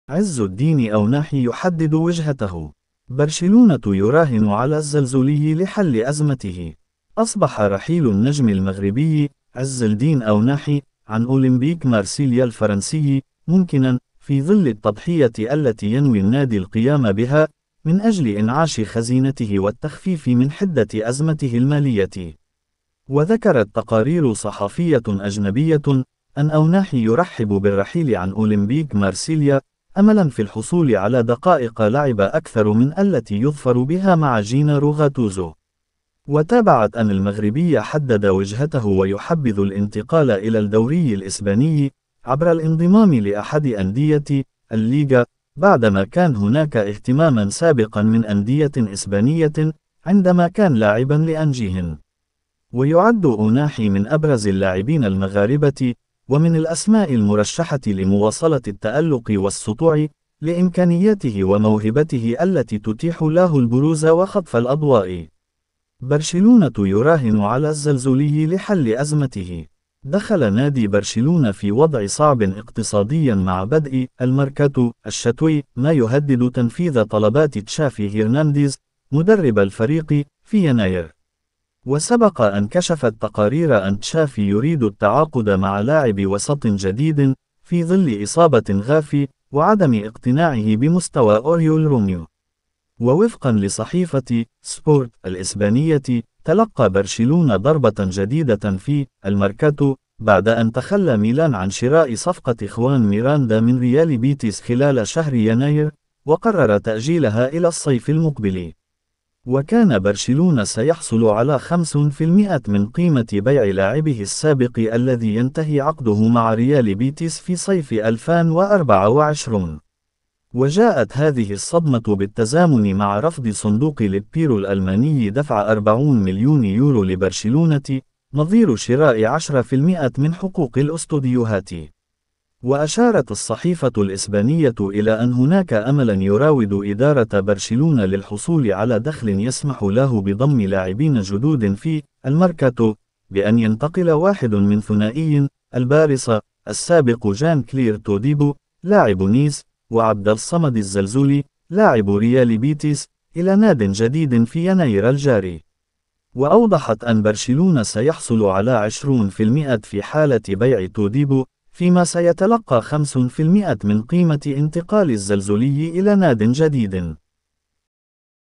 عز الدين أوناحي يحدد وجهته و برشلونة يراهن على الزلزولي لحل أزمته
أًصبح رحيل النجم المغربي، عز الدين أوناحي، عن أولمبيك مارسيليا الفرنسي، ممكنا، في ظل التضحية التي ينوي النادي القيام بها، من أجل إنعاش خزينته والتخفيف من حدة أزمته المالية.
دخل نادي برشلونة في وضع صعب اقتصاديا مع بدء “المركاتو” الشتوي، ما يهدد تنفيذ طلبات تشافي هيرنانديز، مدرب الفريق، في يناير.